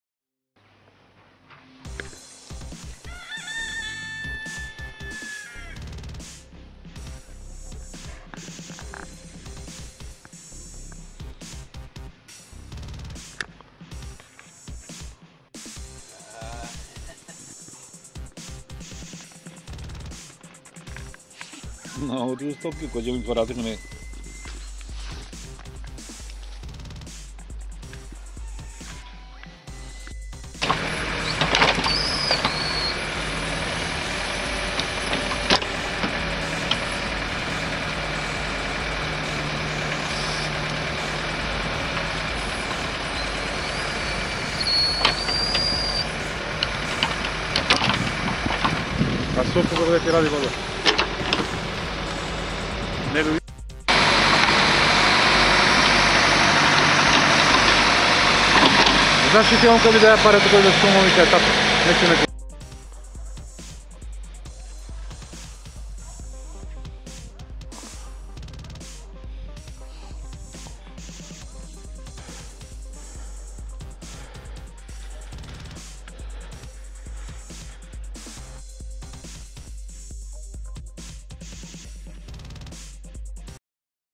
RIBNJAK 1 Belanovce
ribnjak 10m x 10m  sa dubinom od 2m plus 1m bedem-zid bazena